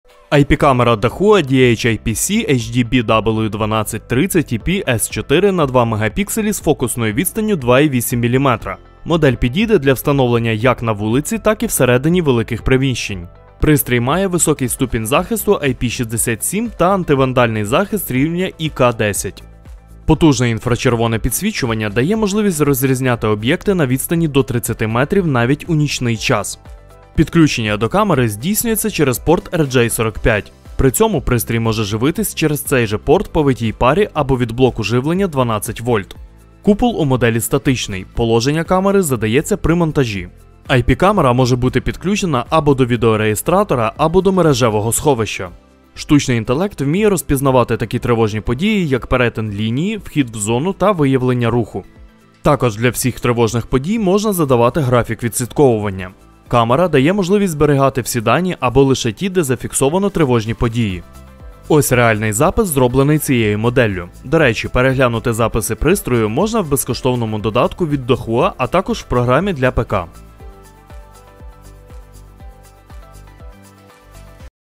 IP-Камера Dahua DH-IPC-HDBW1230EP-S4 (2.8)
✧ IP-Камера на 2 Мп з фокусною відстанню 2,8 мм.
✧ Високий ступінь захисту IP67 та антивандальний захист рівня ІК10
✧ Потужне інфрачервоне підсвічування дає можливість розрізняти об’єкти на відстані до 30 метрів.
✧ Підключення до камери через порт RJ-45.
✧ Пристрій може живитися по витій парі або від блоку живлення 12 В.
✧ Штучний інтелект вміє розпізнавати такі тривожні події, як перетин лінії, вхід в зону та  виявлення руху.